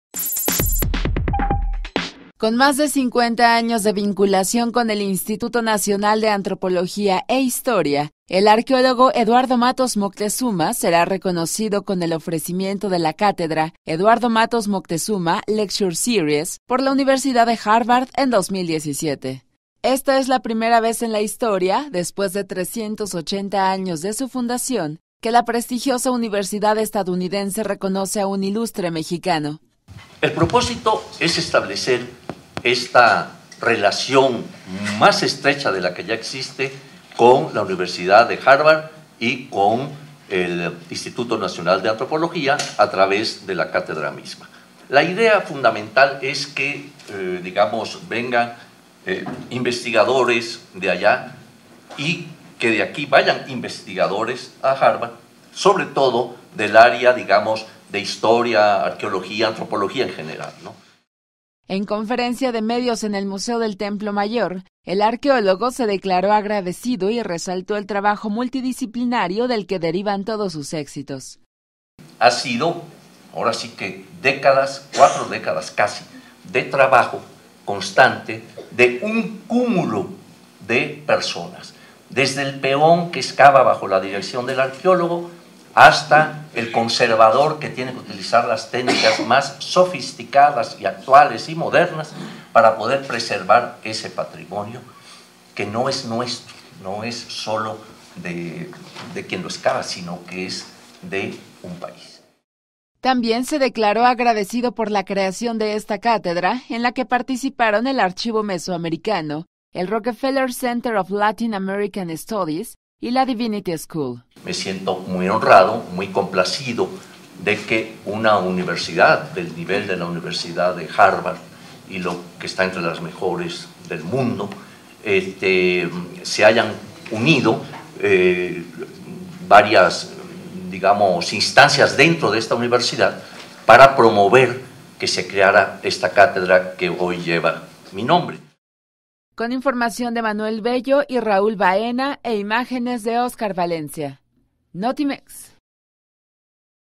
Cátedra Eduardo Matos Moctezuma en Harvard, homenaje a un ilustre mexicano
Con más de 50 años de vinculación con el Instituto Nacional de Antropología e Historia, el arqueólogo Eduardo Matos Moctezuma será reconocido con el ofrecimiento de una cátedra homónima por la Universidad de Harvard en 2017